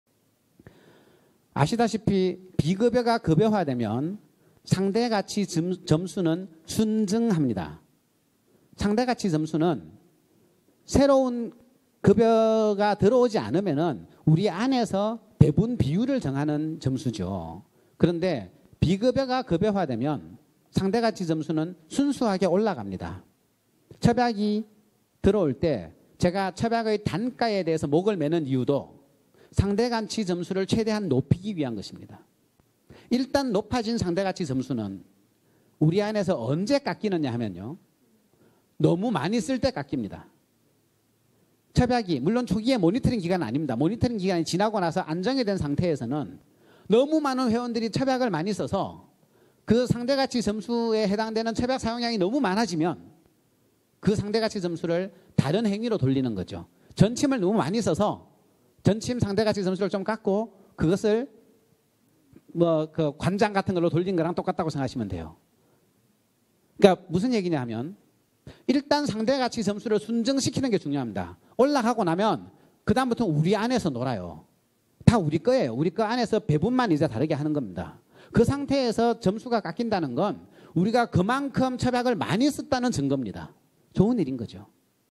10. 첩약건보 첩약 건보의 수가와 상대가치
상대가치점수는 우리 내부에서
각각의 행위의 배분 비율을 결정하는 점수다.
새로운 항목이 급여화 되면, 즉 첩약이 급여화 되면
상대가치점수가 순증한다.

협상에서 수가를 높게 받기 위해서 노력하는 이유도
상대가치점수를 최대한 높이기 위함이고,
일단 높아진 상대가치점수는 사용량이 급증하면 줄어들 수는 있지만, 그 줄어든 만큼 다른 항목으로 이동하는 방식으로 보존된다.
전침의 사용량이 급증하면 전침의 상대가치 점수가 줄어들지만,
그 줄어든 만큼 다른 항목으로 이동하는 것과 같다.